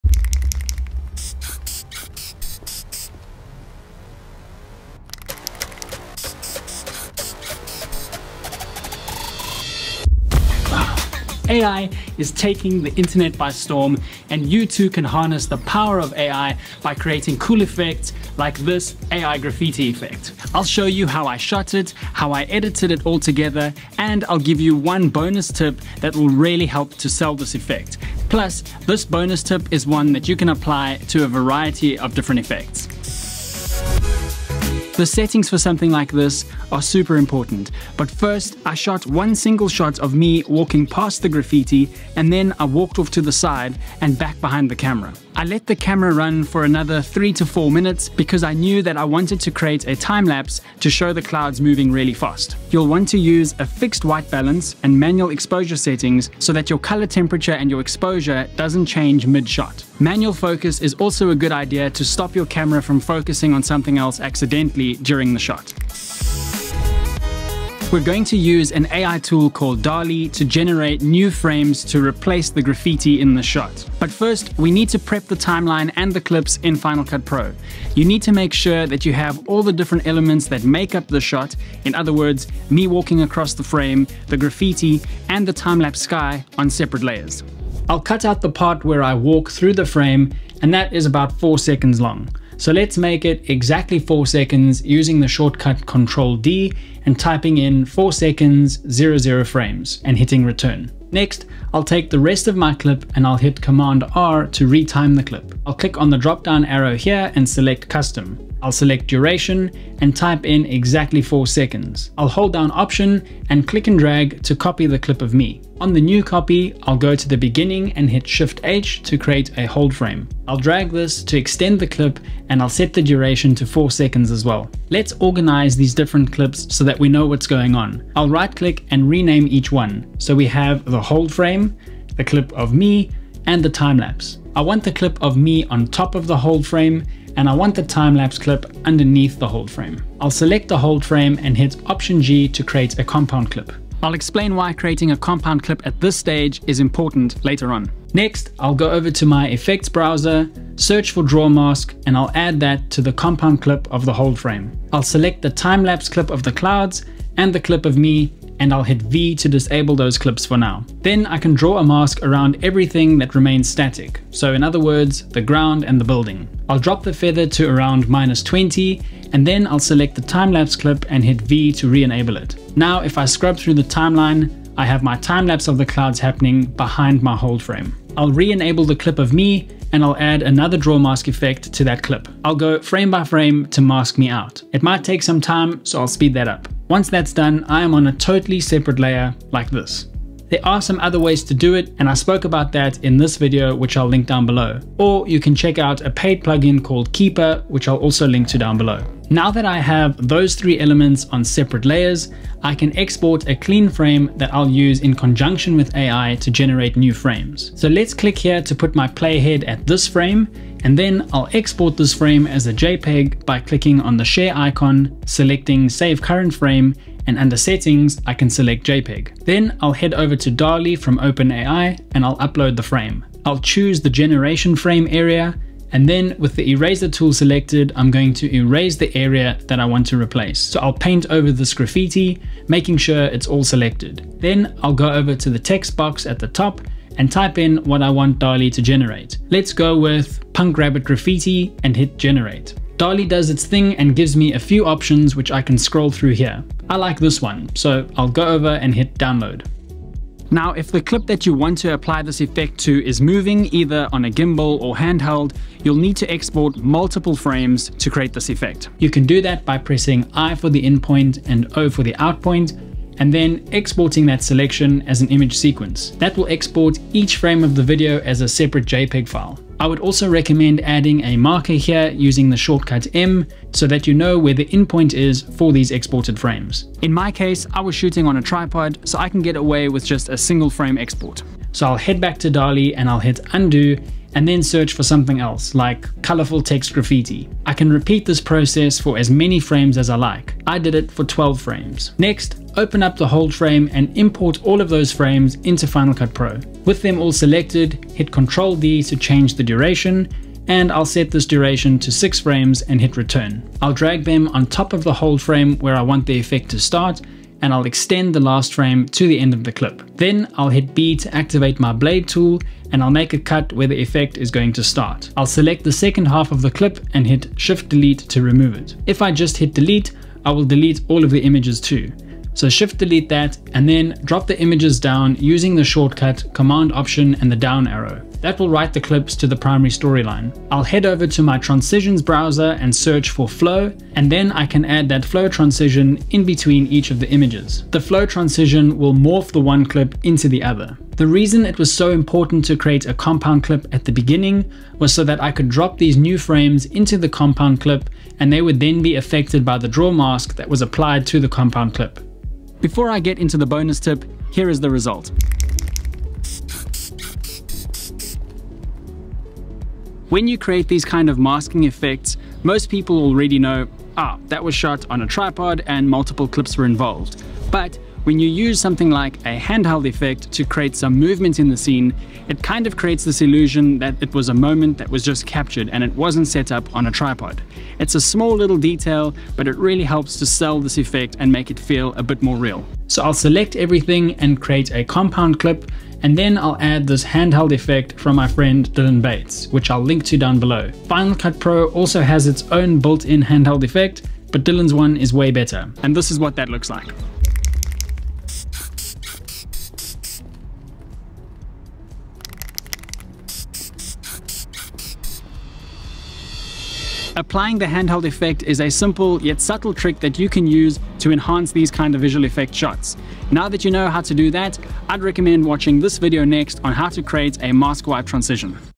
How to Create the AI Graffiti Effect
AI is taking the internet by storm and you too can harness the power of AI to create really cool video effects using Final Cut Pro like this AI Graffiti effect.

I’ll tell you how I shot it, how I edited it all together and one bonus tip that really sells this effect, but also, the bonus tip is one you can apply to a variety of other effects.

When it comes to shooting for the AI Graffiti Effect, the settings are super important, but first, I shot one single shot of me walking past the graffiti and then I walked off to the side and back behind the camera. I let the camera run for another 3-4 minutes because I knew I wanted to create a Timelapse to show the clouds moving really fast. You’ll want to use a fixed white balance and manual exposure settings so that your color temperature and exposure doesn’t change mid shot. Manual focus is also a good idea to stop your camera from accidentally focusing on something else.

We’re going to use an AI tool called Dall-E to remove parts of the frame and generate new graffiti but first we need to prep the timeline and clips. You need to have each of the different elements that make up this shot, in other words, me walking through the frame, the Timelapse sky and the graffiti on separate layers.

🔴 CHAPTERS 🔴
00:00 - How to Create the AI Graffiti Effect
00:33 - Shooting for the AI Graffiti Effect
01:06 - Editing the AI Graffiti Effect
05:59 - Bonus Tip